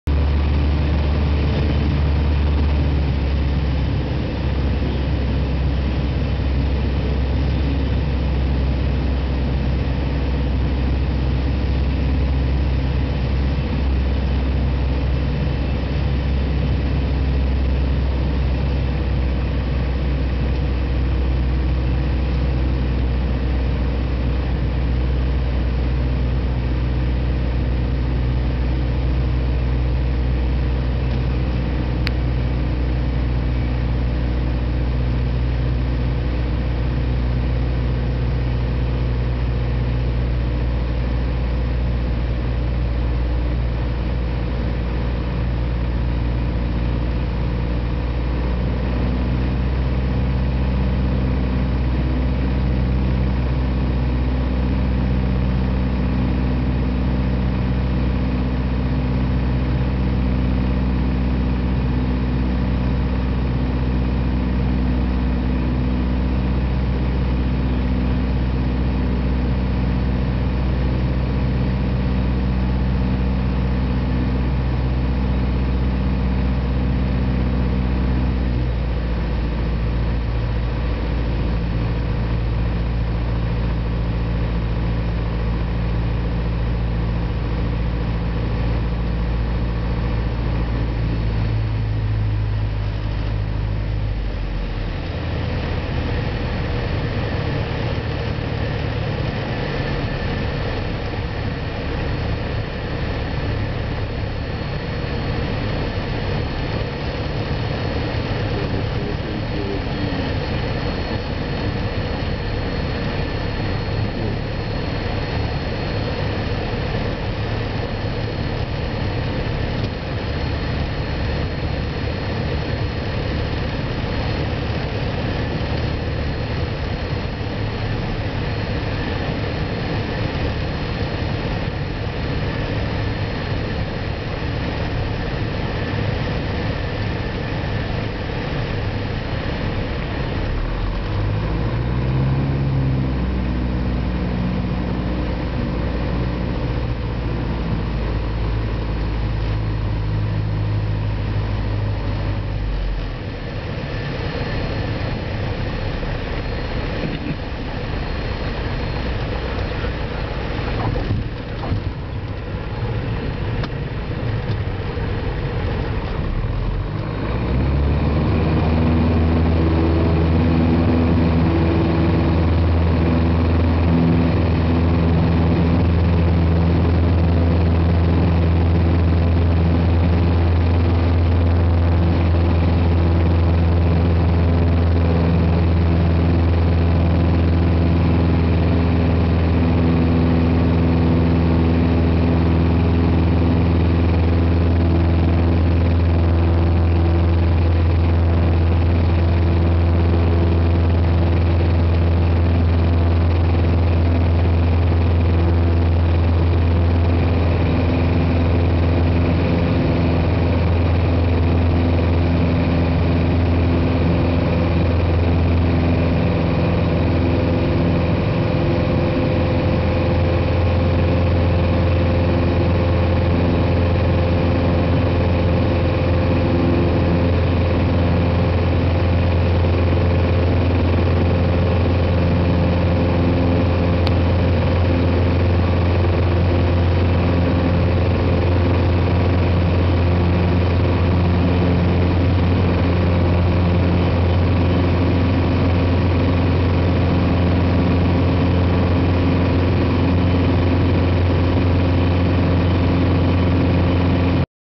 The Dallas, OR, KDLS, rwy 31 touch & go Piper PA 28 Cherokee, 06-01-2014
Wind: 310°, 13 kts